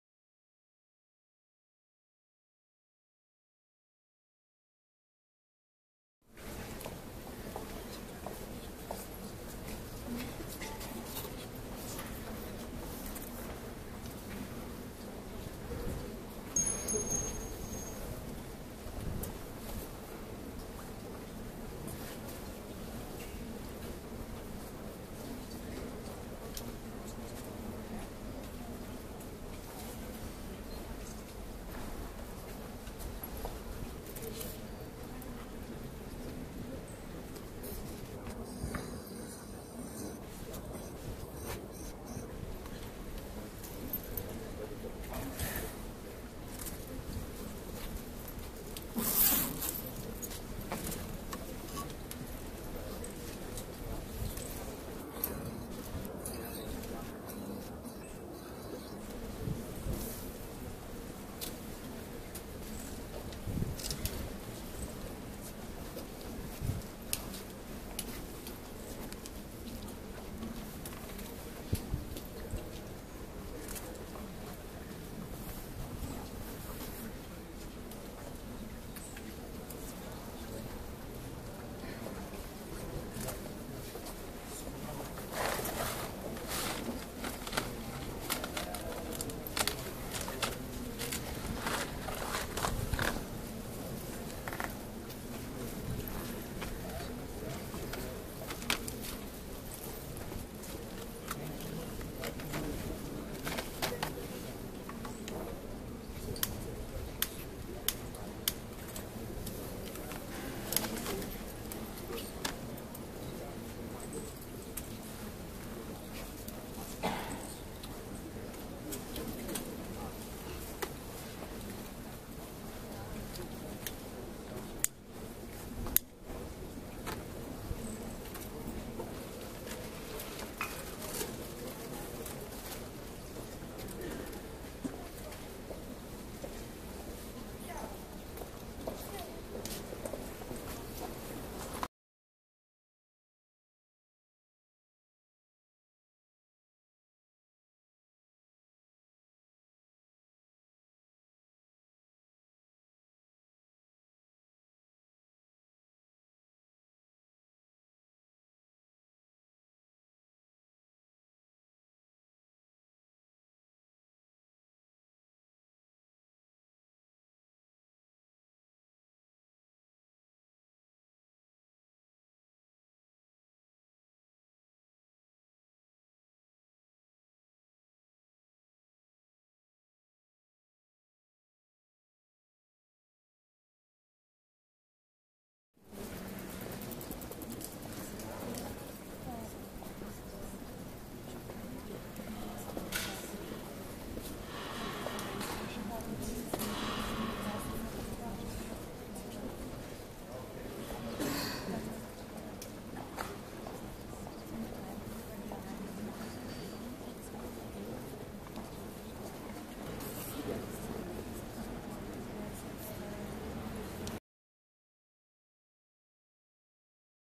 On the Grid Ep. 10, Kerzenmeer in Graz nach Amokfahrt
Tausende Grazer und Bürger aus aller Welt teilten am Abend des 20. Juni ihre Trauer in der Grazer Innenstadt. An den Unfallstellen wie auch bei der Polizeistation in der Schmidtgasse wurden Kerzen und Blumen hingelegt.

Im Bild ist die Unfallstelle Albrechtgasse/Schmidtgasse/Hauptplatz zu sehen.